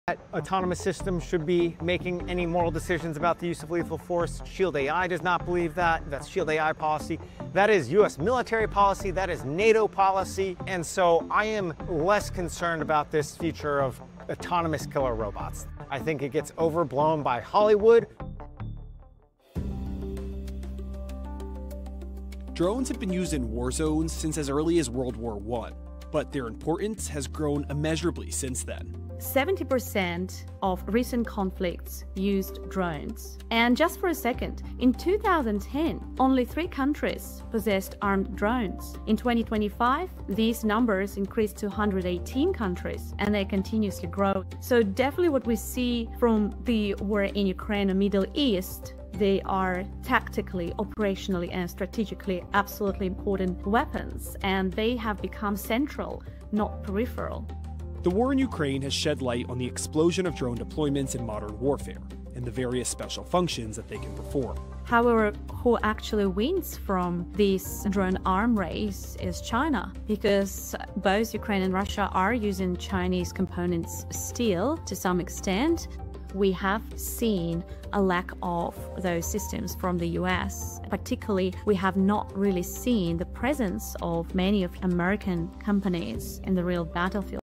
Drones: From World War I to Modern Warfare's Central Weapon
AI and drones reshape modern warfare dynamics.